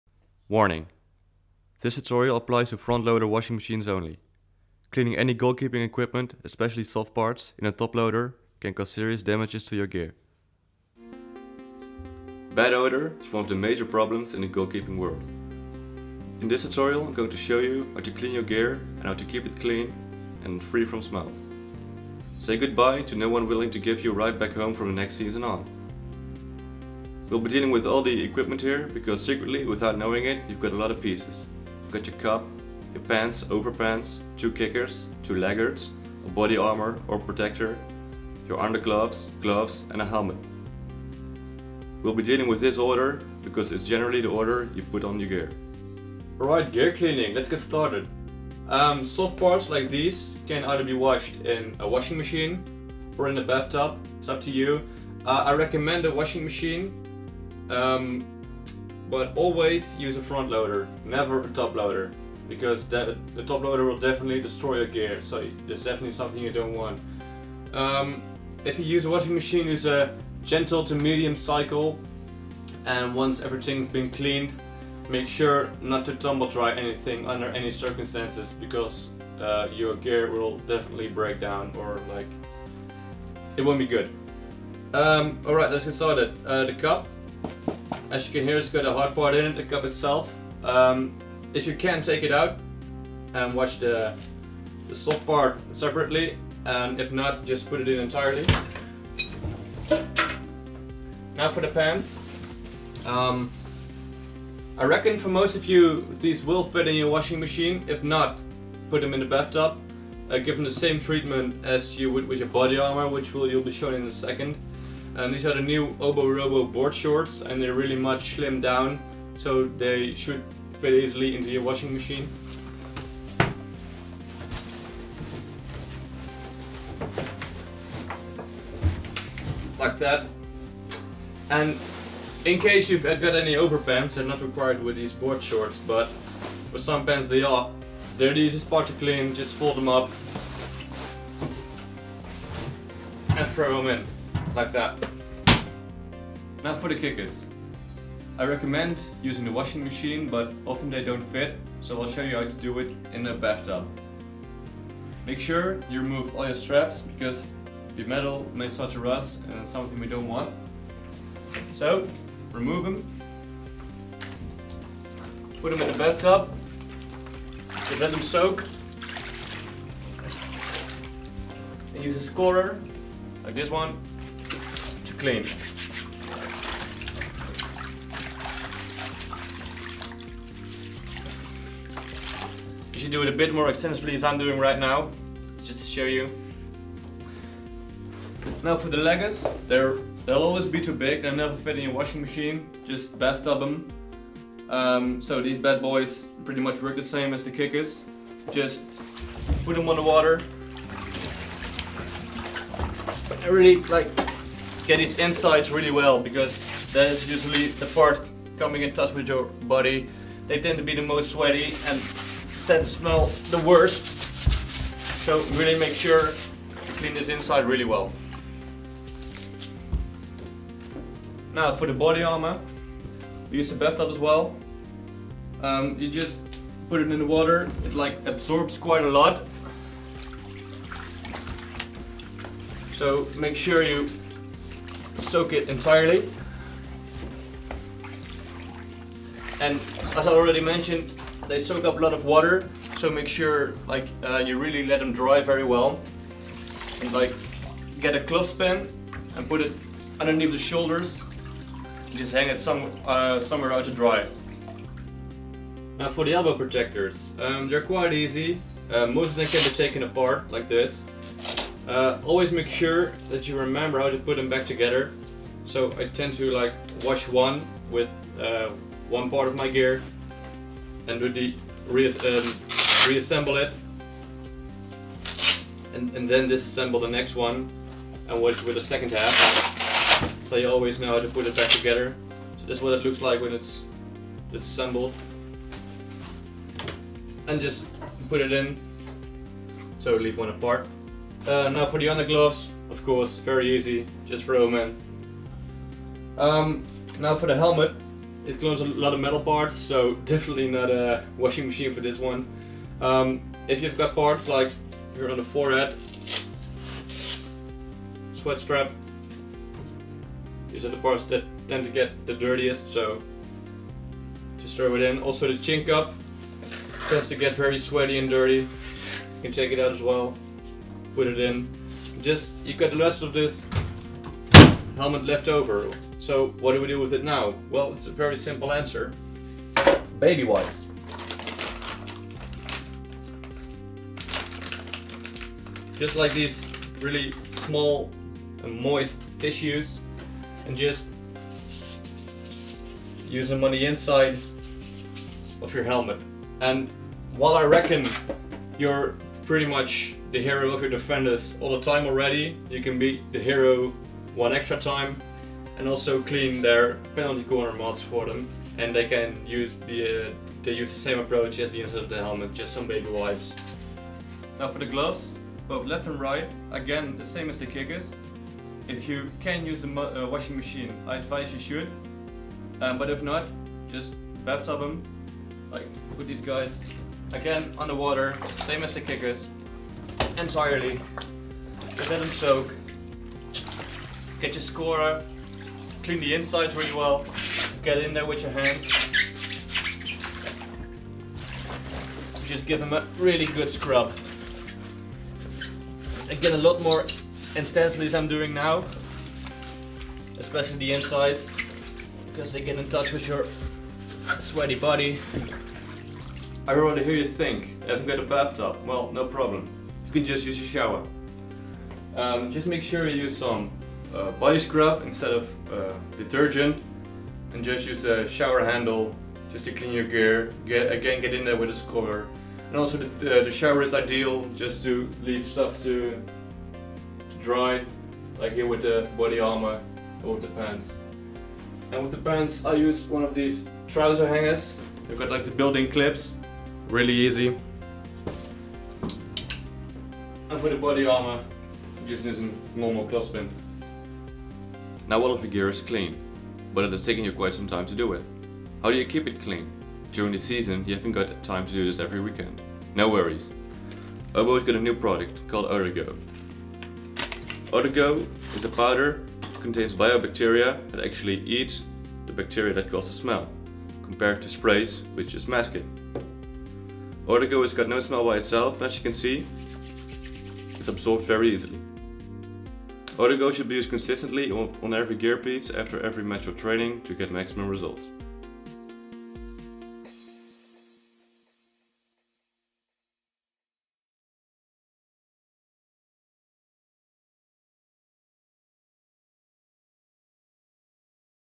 OBO Tutorial Gear Cleaning
Learn the best way to make your gear smell as good as new again!